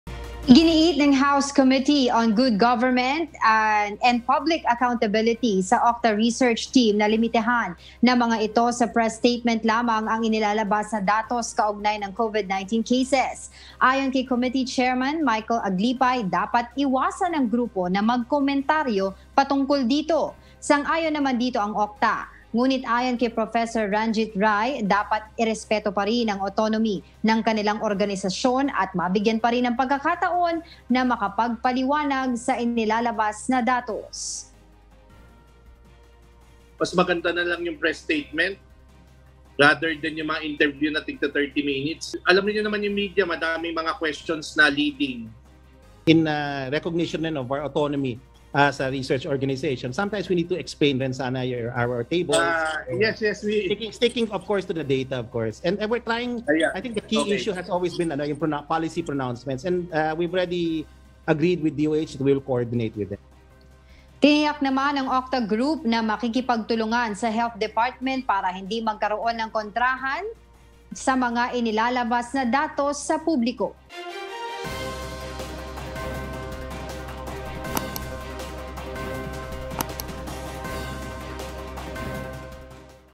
OCTA team, pinaiiwas ng Kamara na magkomentaryo sa COVID-19 data
Iginiit ng House committee on good government and public accountability sa OCTA Research na limitahan sa press statement lamang ang inilalabas na datos kaugnay ng COVID-19 cases.